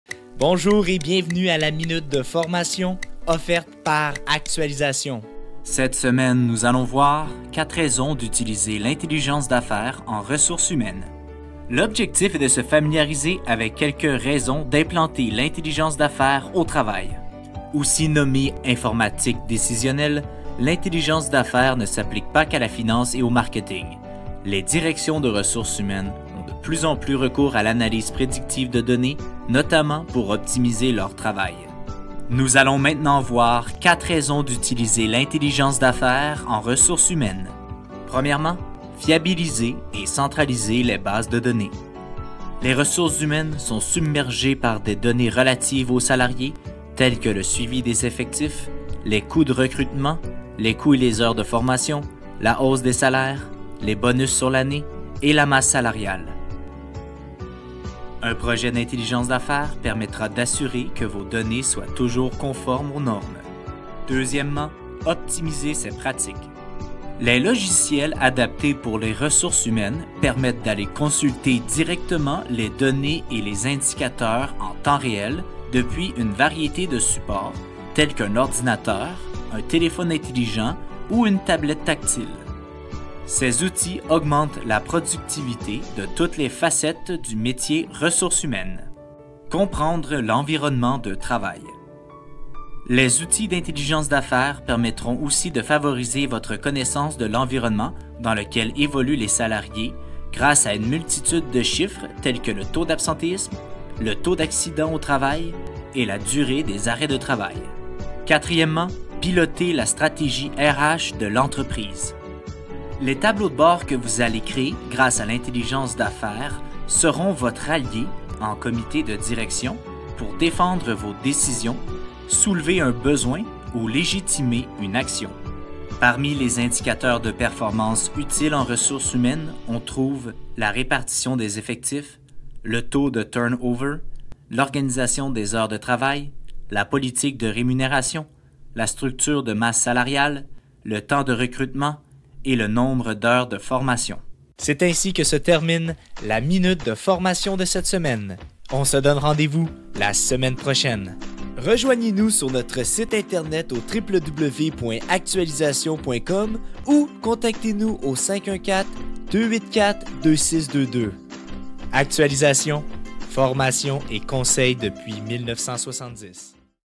4 raisons d'utiliser l'intelligence d'affaires en ressources humaines
Utilisez-vous l'intelligence d'affaires pour optimiser votre travail?

Cette semaine, dans la minute de formation: «4 raisons d'utiliser l'intelligence d'affaires en ressources humaines», nous allons nous familiariser avec quelques raisons d'implanter l'intelligence d'affaires au travail.

L’intelligence d’affaires portant aussi le nom d’informatique décisionnelle, ne s’applique pas seulement à la finance et au marketing. Les directions des ressources humaines ont de plus en plus recours à l’analyse prédictive des données notamment pour optimiser leur travail.

Voici 4 raisons d’utiliser l’intelligence d’affaires en ressources humaines:

1. Fiabiliser et centraliser les bases de données
Les ressources humaines sont submergées par des données relatives aux salariés tels que le suivi des effectifs, les coûts de recrutement, les coûts et heures de formation, la hausse des salaires, les bonus sur l’année et la masse salariale. Un projet d’intelligence d’affaires permettra d’assurer que les données soient toujours conformes aux normes.

2. Optimiser les pratiques
Les logiciels adaptés pour les ressources humaines permettent d’aller consulter directement les données et indicateurs en temps réel depuis une variété de supports tels qu’un ordinateur, un téléphone intelligent ou une tablette tactile. Ces outils augmentent la productivité de toutes les facettes du métier ressources humaines.

3. Comprendre l’environnement de travail
Les outils d’intelligence d’affaires permettront aussi de favoriser votre connaissance de l’environnement dans lequel évoluent les salariés grâce à une multitude de chiffres tels que le taux d’absentéisme, le taux d’accidents au travail et la durée des arrêts de travail.

4. Piloter la stratégie ressources humaines de l’entreprise
Les tableaux de bords que vous allez créer grâce à l’intelligence d’affaires seront votre allié en comité de direction pour défendre vos décisions, soulever un besoin ou légitimer une action. Parmi les indicateurs de performance utiles en ressources humaines nous pouvons trouver : la répartition des effectifs, le taux de turn-over, l’organisation des heures de travail, la politique de rémunération, la structure de masse salariale, le temps de recrutement et le nombre d’heures de formation.

On se donne rendez-vous la semaine prochaine!